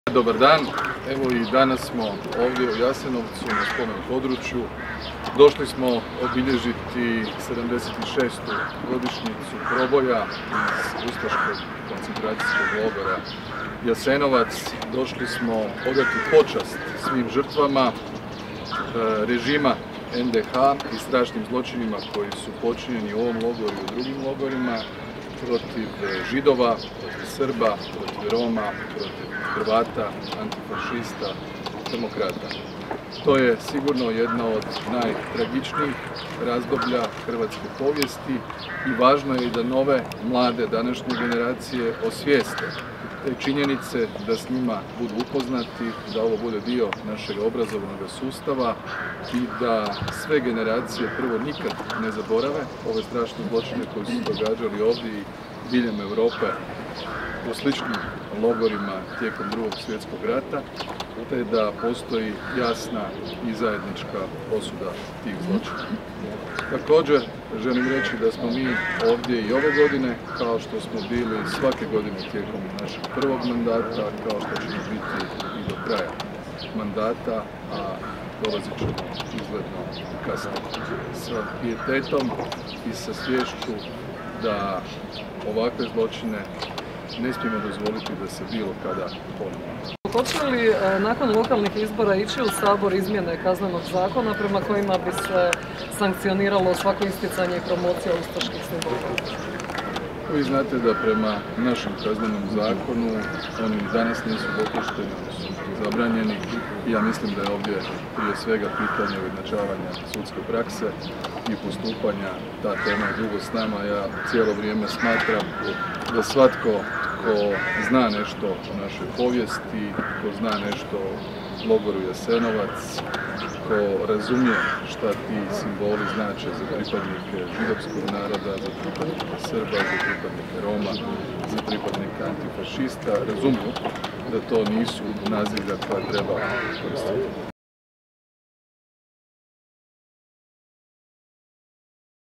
Premijerj Plenković o ustaškom režimu u znakovlju HOS-a
Povodom 76. obljetnice proboja posljednjih zatočenika ustaškog logora Jasenovac, izaslanstva Predsjednika, Sabora i Vlade, kao i manjina, odali su počast svim stradalima u tom logoru polaganjem cvijeća i kamenčića kod Spomen obilježja Kameni cvijet.